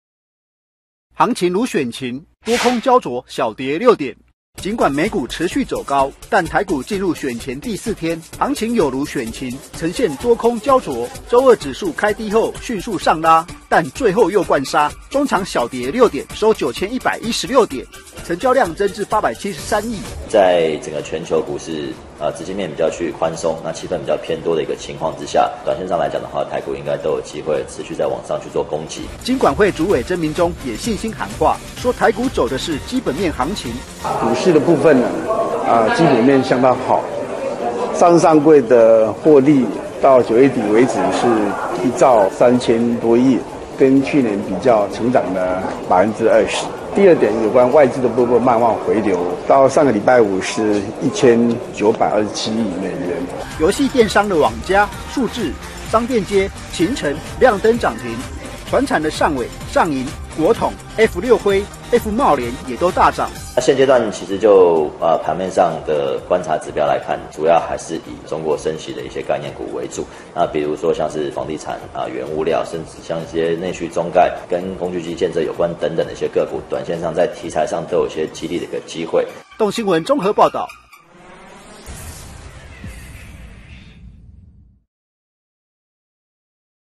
行情如選情 多空膠著小跌6點 --蘋果日報 20141126
【王立德╱台北報導】央行喊貨幣政策轉「中性」不是喊假的，央行昨日公布10月金融情況，日均M1B、M2的年增率雙雙下滑至7.18%、4.98%，其中M2年增率更創16個月來新低，也是16個月來首見「4字頭」，台灣資金面湧現大退潮。
M1B年增率也上演連3降；M2則是連2降，不過央行經濟研究處副處長吳懿娟強調，前10月M2平均年增率5.69%，資金動能仍足以支應經濟活動所需。
吳懿娟解釋，10月M1B、M2年增率雙降，主因為股市較不活絡、證券劃撥存款減少，及外資單月淨匯出12.8億美元所導致，此外，去年基期較高也有影響。

行情如選情
多空膠著小跌6點
儘管美股持續走高
但台股進入選前第4天
行情有如選情
呈現多空膠著
周二指數開低後
迅速上拉 但最後又摜殺
終場小跌6點 收9116點
成交量增加至873億

在整個全球股市
資金面比較趨於寬鬆
氣氛比較偏多的一個情況之下
短線上來講的話
台股應該都有機會
持續再往上去做攻擊

金管會主委曾銘宗也信心喊話
說台股走的是「基本面行情」

股市的部分 基本面相當好
上市上櫃的獲利到9月底為止
是1兆3000多億
跟去年比較 成長了20%
第2點有關外資的部分 慢慢回流
到上個禮拜五是1927億美元

遊戲電商的網家 數字
商店街 勤誠 亮燈漲停
傳產的上緯 上銀 國統
 F-六暉 F-貿聯也都大漲

現階段其實就…盤面上的
觀察指標來看
主要還是以中國升息的
一些概念股為主
比如說像是房地產 原物料
甚至像一些內需中概
跟工具機建設有關
等等的一些個股
短線上在題材上
都有一些激勵的機會

動新聞綜合報導